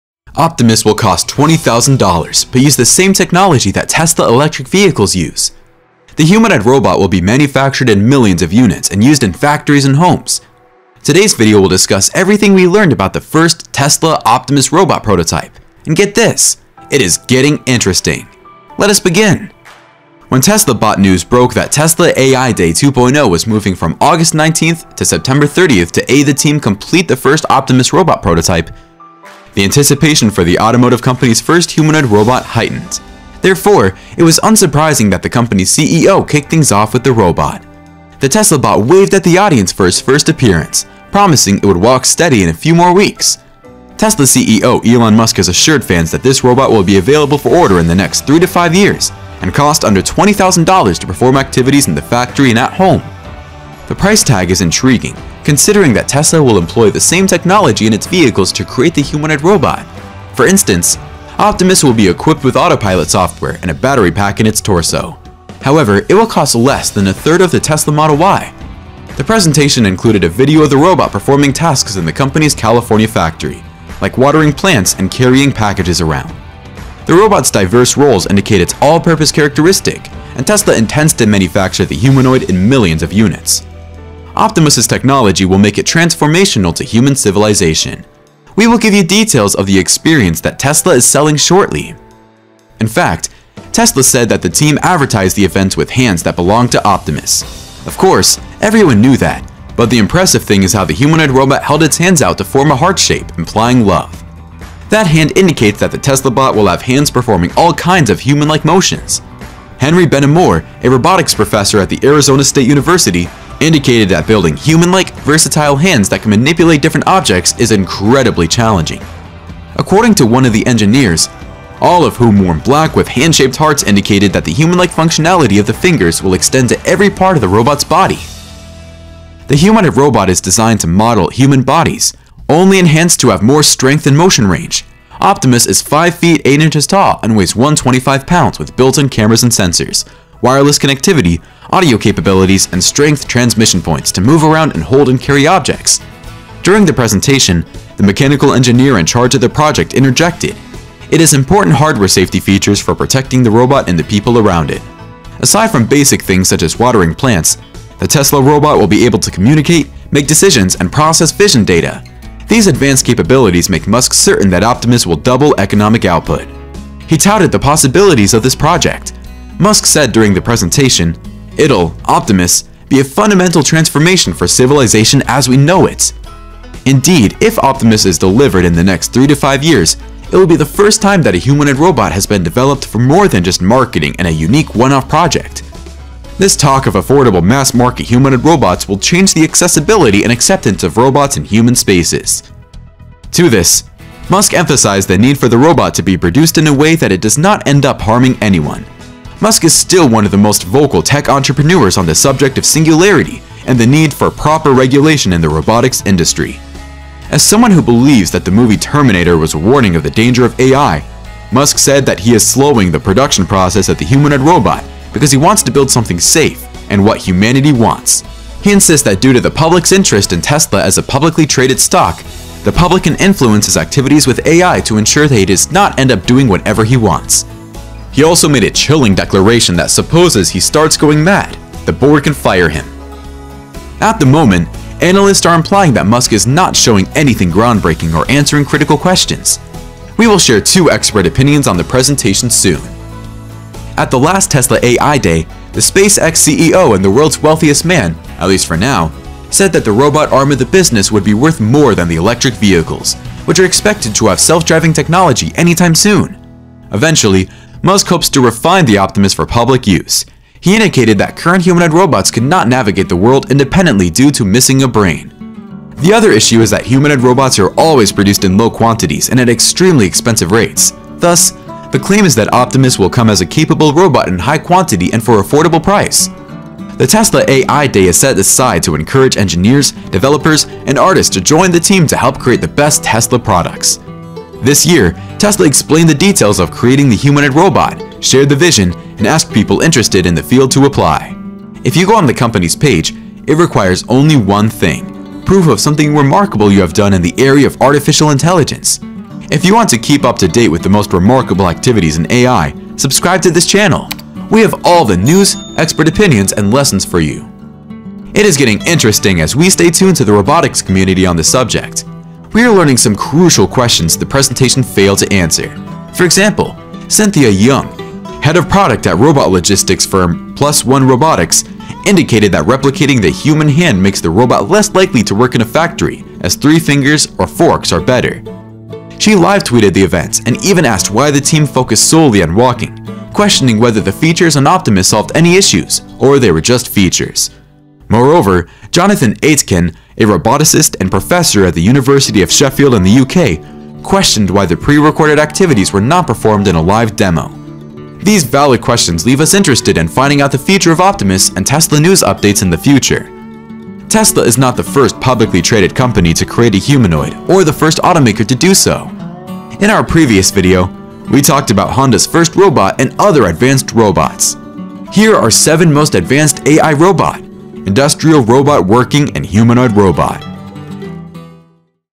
Tesla Optimus Robot is Finaly Here: This is Getting Interesting
Telsa Optimus Robot is Finaly Here: This is Getting Interesting. Tesla bot features drew lot of design inspiration from the human body. Not surprising tesla optimus has a human shape. Elon musk reveals tesla bot finally during the tesla ai day 2022 and it has sparked lot of conversation. With many wondering the capabilities of tesla robot. Tesla news about the optimus robot keeps getting interesting.

Optimus will cost $20,000 but use the same technology that Tesla electric vehicles use. The humanoid robot will be manufactured in millions of units and used in factories and homes. Today’s video will discuss everything we learned about the first Tesla Optimus Robot prototype. And, get this, it is getting interesting! Let us begin

Timestamps
00:00 Introduction
Card: 7 Most Advanced AI Robot: Industrial Robot Working & Humanoid Robot